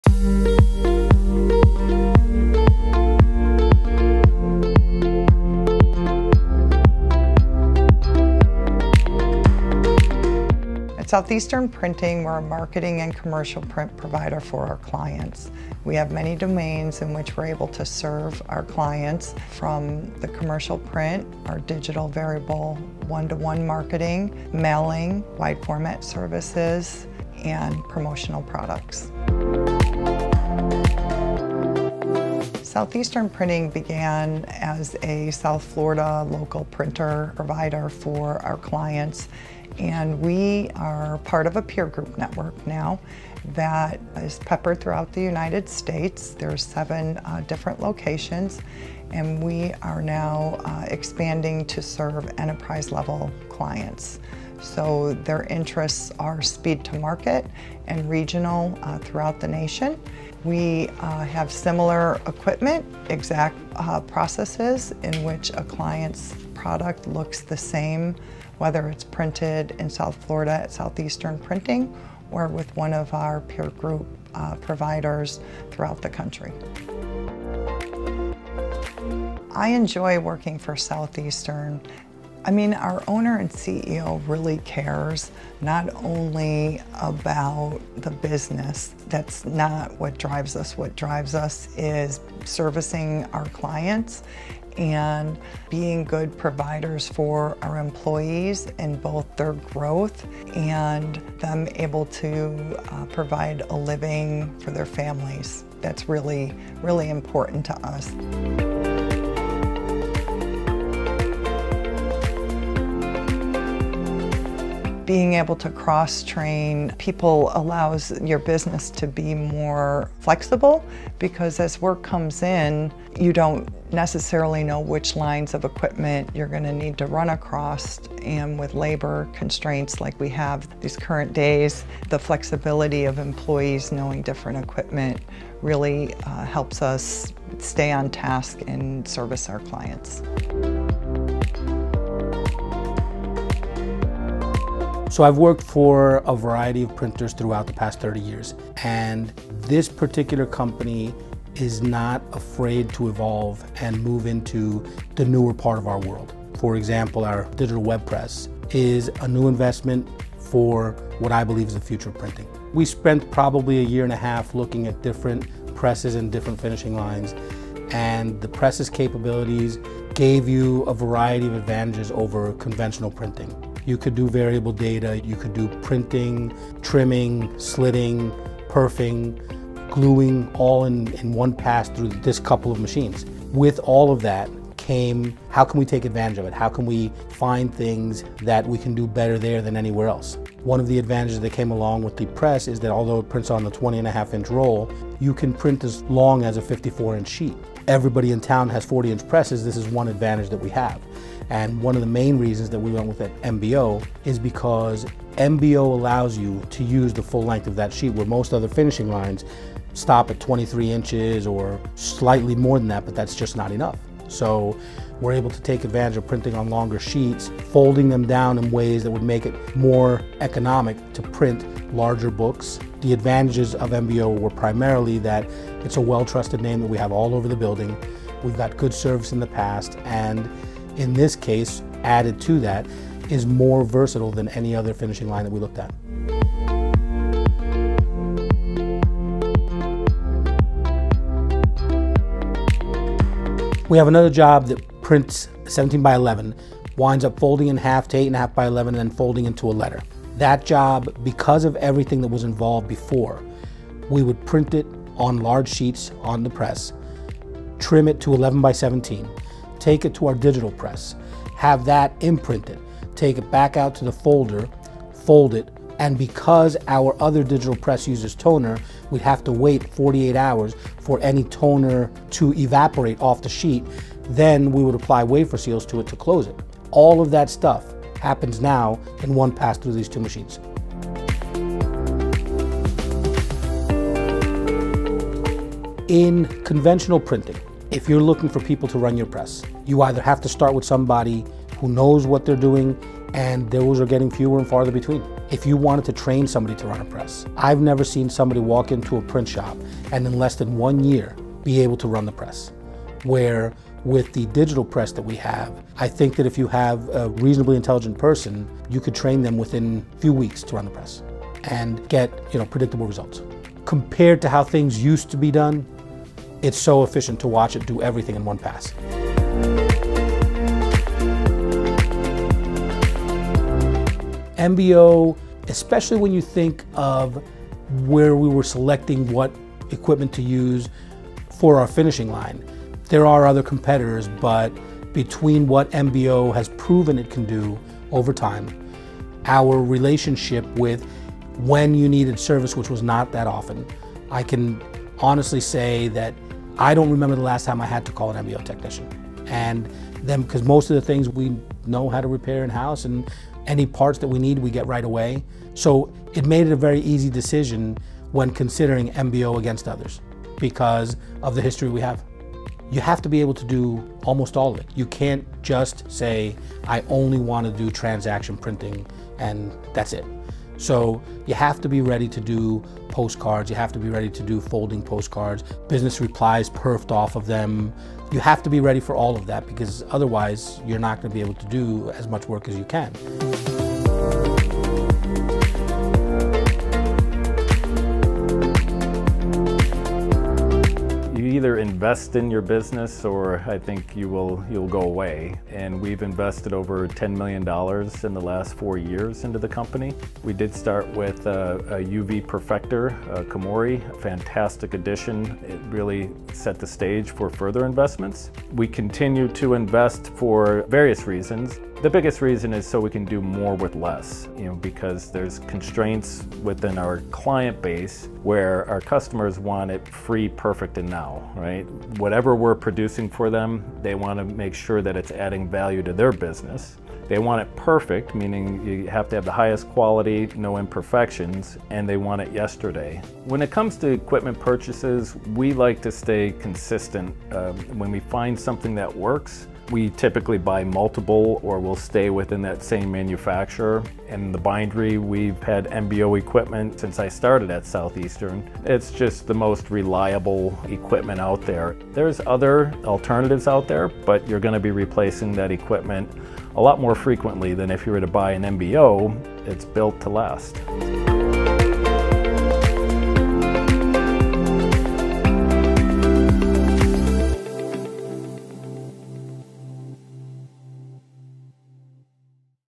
Southeastern Printing discusses their business and relationship with Komori and MBO
Southeastern has invested millions in the last 2-3 years to upgrade their production processes to meet the needs of the new print industry. It started with the addition of a Komori GL840P sheetfed perfector offset press, which paved the way for a new Screen 520HD inkjet press, MBO Rollfed Finishing System and Muller Martini Presto II Digital Stitcher. These investments increase production and reduce the dependency upon labor, setting the company up for success! Catch the comments from Don Mader-CEO, Christine Bitner-President, and Vince Pilla-Director of Variable Print Ops, on their new business model and partnership with MBO and Komori.